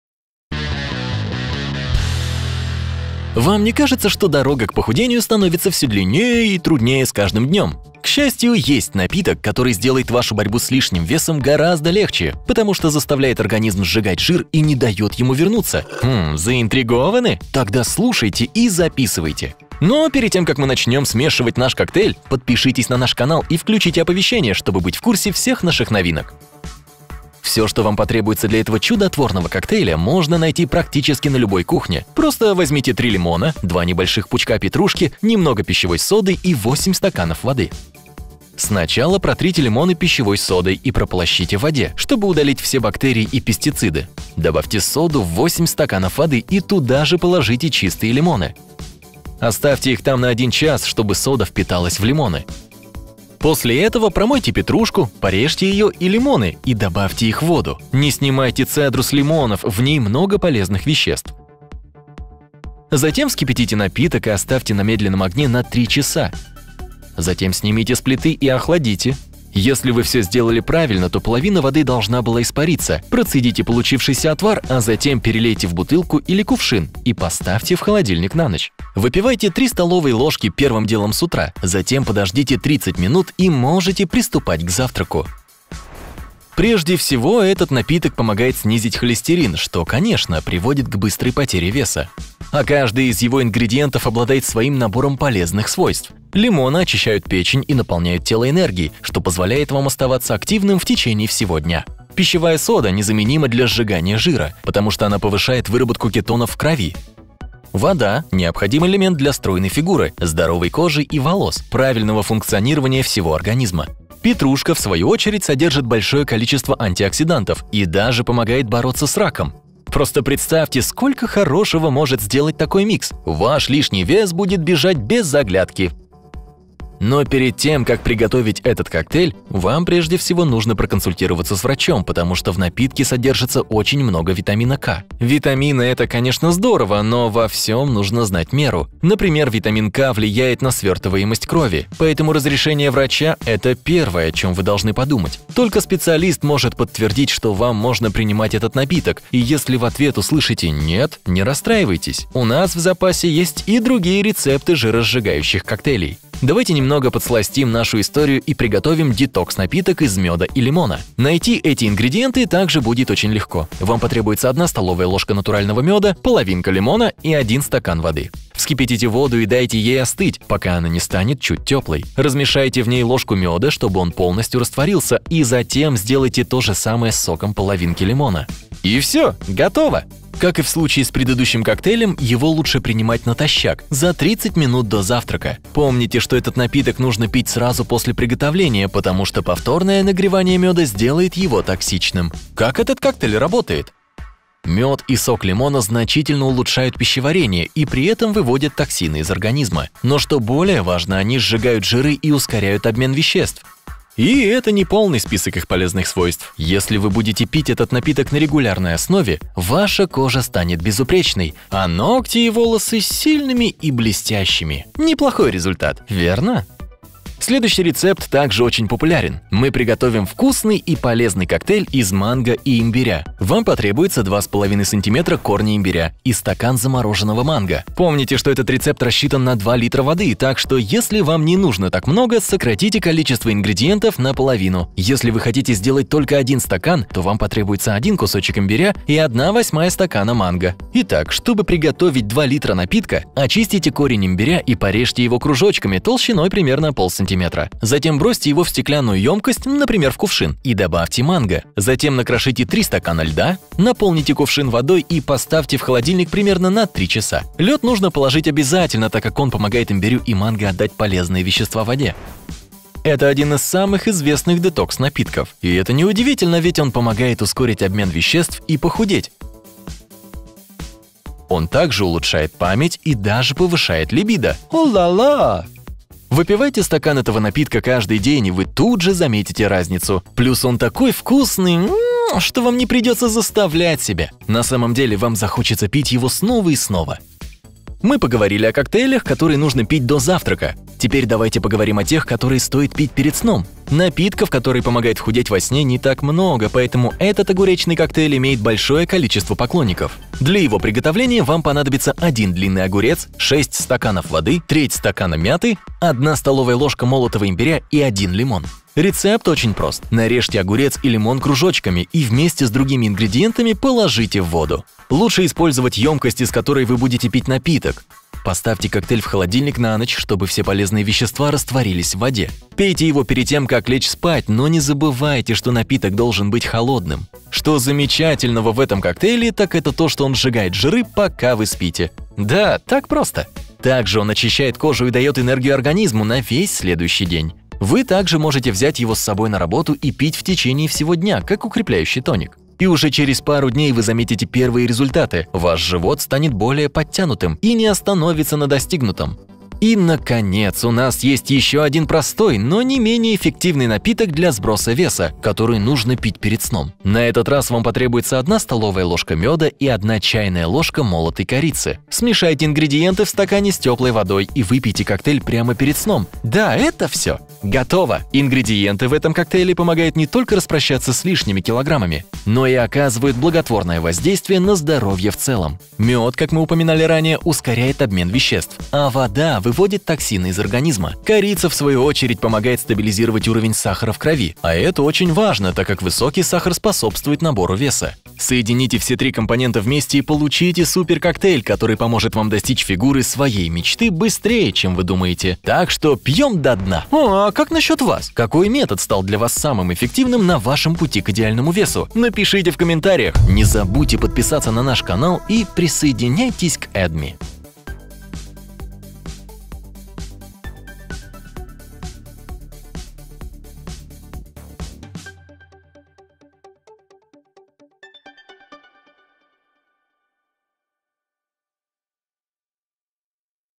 Этот Напиток Поможет Вам Избавиться от Жира на Животе, Руках и Ногах Всего за 1 Неделю
Вам не кажется, что дорога к похудению становится все длиннее и труднее с каждым днем? К счастью, есть такой напиток, который сделает вашу борьбу с лишним весом гораздо легче, потому что заставляет организм сжигать жир и не дает ему вернуться. Заинтригованы? Тогда слушайте и записывайте.
Все, что вам потребуется для этого чудотворного коктейля, можно найти практически на любой кухне. Просто возьмите 3 лимона, 2 небольших пучка петрушки, немного пищевой соды и 8 стаканов воды. В начале протрите лимоны пищевой содой и прополощите в воде, чтобы удалить все бактерии и пестициды. Добавьте соду в 8 стаканов воды и туда же положите чистые лимоны. Оставьте их на час, так чтобы сода впиталась во фрукты. После этого промойте петрушку, порежьте ее и лимоны и бросьте их в воду.
Не снимайте цедру с лимонов, в ней много полезных веществ. Затем вскипятите воду и оставьте на медленном огне на 3 часа. Затем снимите с плиты и охладите. Если вы все сделали правильно, то половина воды должна была испариться. Процедите получившийся отвар, а затем перелейте в бутылку или кувшин и поставьте в холодильник на ночь. Выпивайте 3 столовые ложки первым делом с утра, затем подождите 30 минут и можете приступать к завтраку.